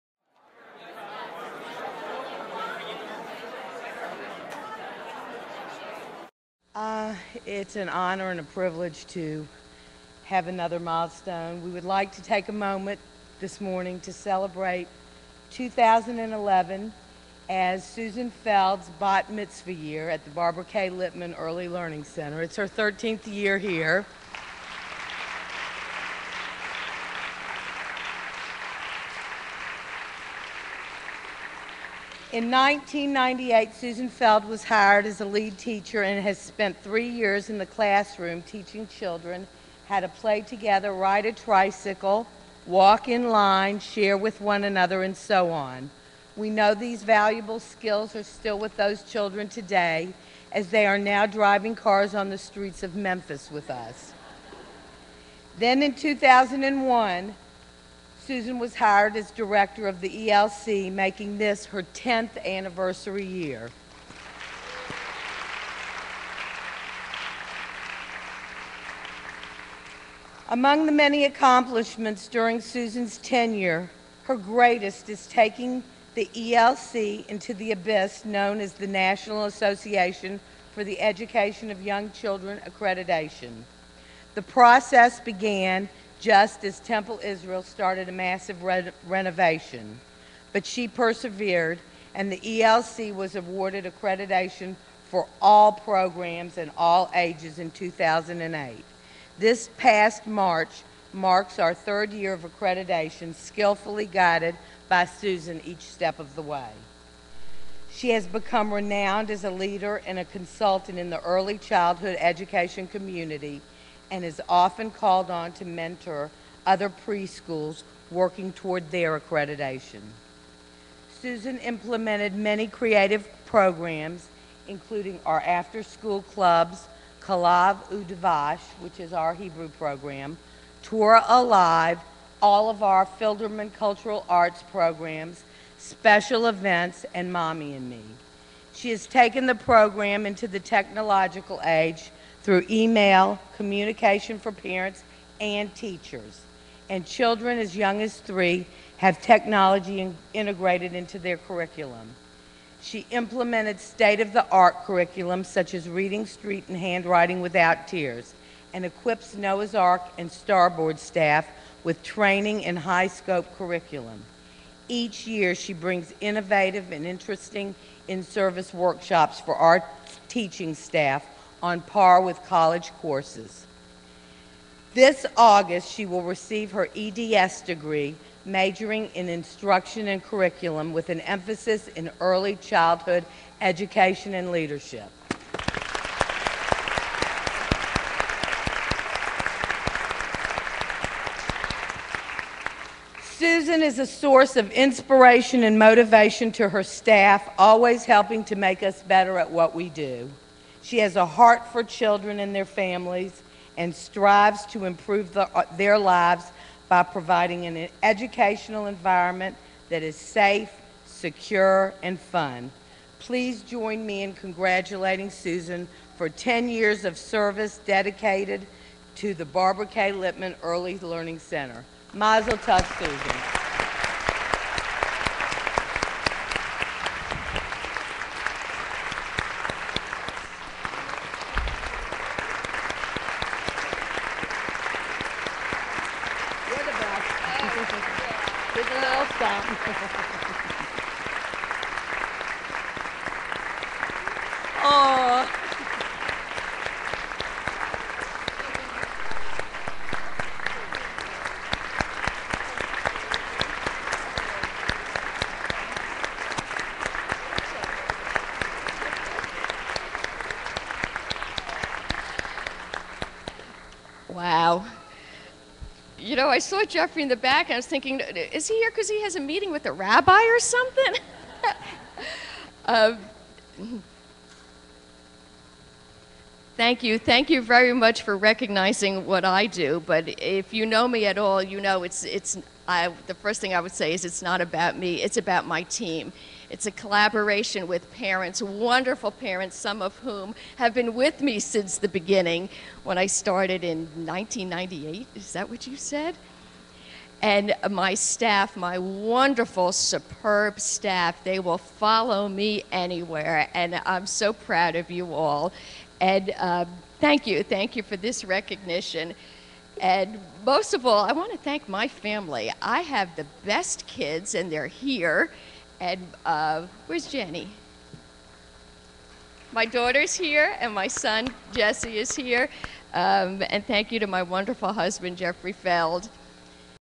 Susan Feld Honored
Susan Feld, Director of the Barbara K. Lipman Early Learning Center at Temple Israel in Memphis, was honored during the preschool's Kindergarten Shalom Ceremony May 27, 2011.